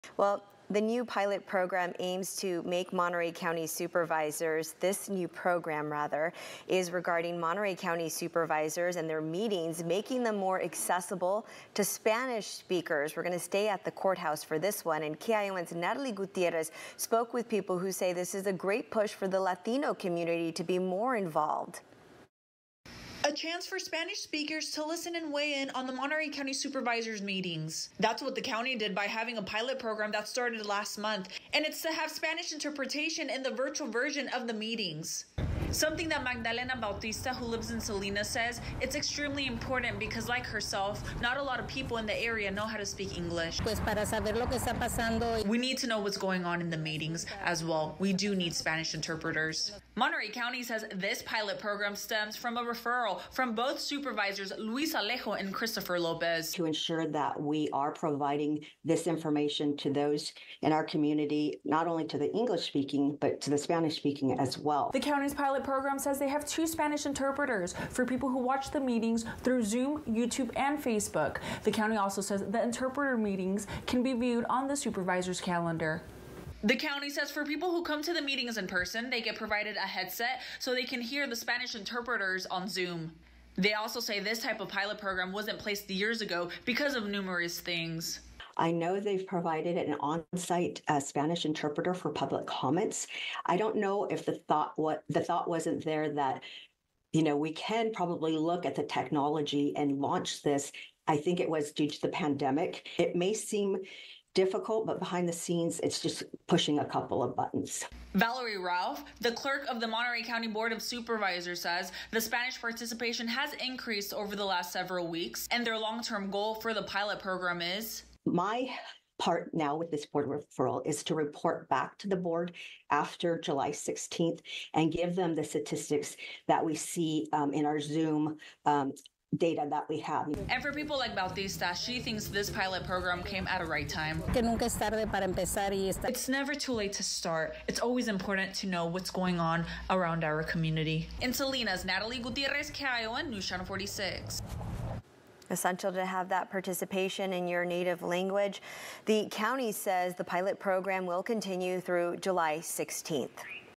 Monterey County pilot program gives Spanish speakers an opportunity to be involved in the ...
Monterey County pilot program gives Spanish speakers an opportunity to be involved in the Supervisors meetings | Read the full story